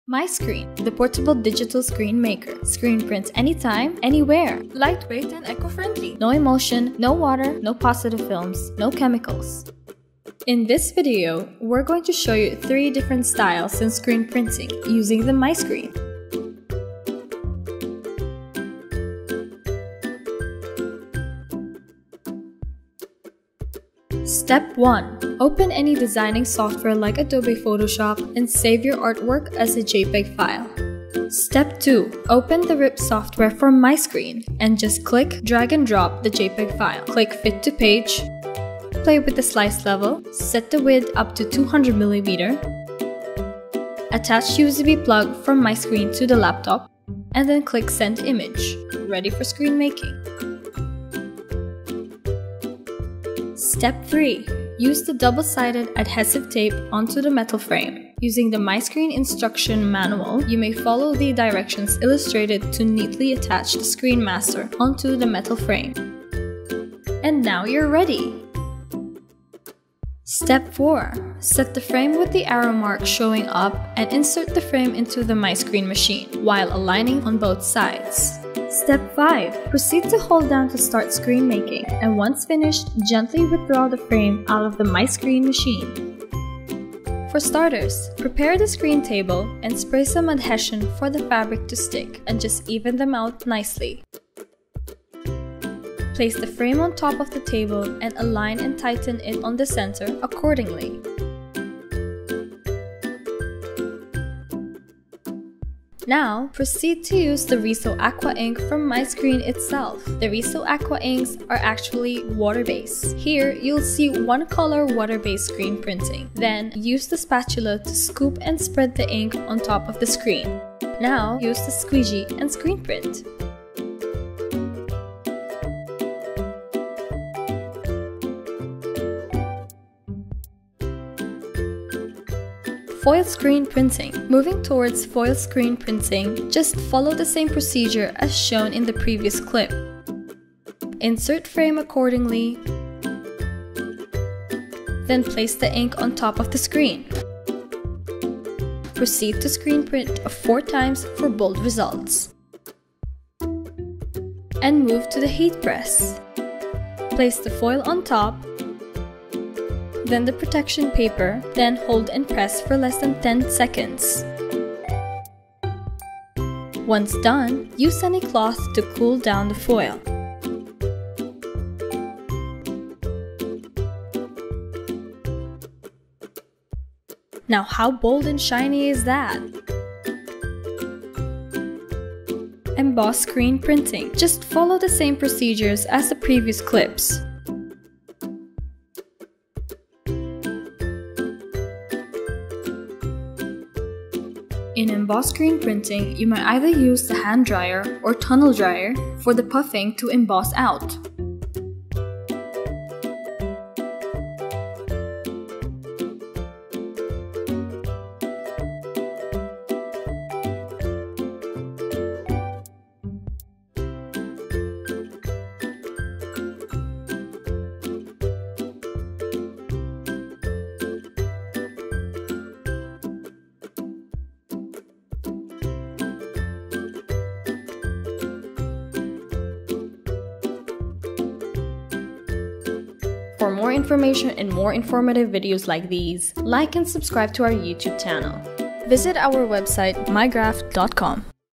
MiScreen: Screen Printing Applications - Digital Screen Maker
MiScreen, the portable digital screen maker. Screen print anytime, anywhere! Lightweight and eco friendly, no emulsion, no water, no positive films, no chemicals.

Watch three of the many ways to diy screen printing with MiScreen, the first and the latest portable and compact digital screen maker, expose your screen within seconds and screen print anywhere, fast and easy. You may instantly customize on any materials with MiScreen, use the RISO Aqua Inks – water based screen printing inks included in the MiScreen Box, go bold and shiny with your screen printing results with foil printing, add intensity in your print outcomes with emboss screen printing.

In this video, were going to show you 3 different styles in screen printing using the MiScreen.

Step 1 - Open any designing software like Adobe Photoshop and save your artwork as a jpeg file

Step 2 - Open the rip software for MiScreen, and just click - drag - and drop the jpeg file, click fit-to-page, set the width upto 200mm, proceed to play with the Slice level, attach usb plug from MiScreen to the laptop, and then click send image. Thus, having the MiScreen ready for screen making. The rip software is compatible with MacOS and Windows.

Step 3 - Use the double sided adhesive tape onto the metal frame, using the MiScreen Instruction manual you may follow the directions illustrated to neatly attach the screen master onto the metal frame.

And now you’re ready!

Step 4 - set the frame with the arrow mark showing up and insert the frame into the MiScreen Machine while aligning on both sides.

Step 5 - proceed to hold down to start screen making, and once finished gently withdraw the frame out of the MiScreen Machine.

Screen printing time!
Prepare the screen table and spray some adhesion for the fabric to stick and just even them out nicely. Place the frame on top of the table and align and tighten it on the center accordingly. Now, proceed to use the RISO Aqua ink from MiScreen itself, the RISO Aqua inks are water base, here you’ll see 1 COLOR WATER BASED SCREEN PRINTING. Then, use the spatula to scoop and spread the ink on top of the screen. Now you may proceed to use the squeegee and screen print!

Foil screen printing, moving towards foil screen printing. Follow the same procedure as shown in previous clip, insert frame accordingly, place the ink on top of the screen, screen print 4 times for bold results, and move to the heat press, place the foil on top, then the protection paper hold and press for less than 10 secs. Once done, use any cloth to cool down the foil for a bit and then proceed to remove the foil.

How is bold and shiny is that?

Emboss screen printing, follow the same procedures as the previous clips. In emboss screen printing you may use hand dryer or tunnel dryer for the puff ink to emboss out.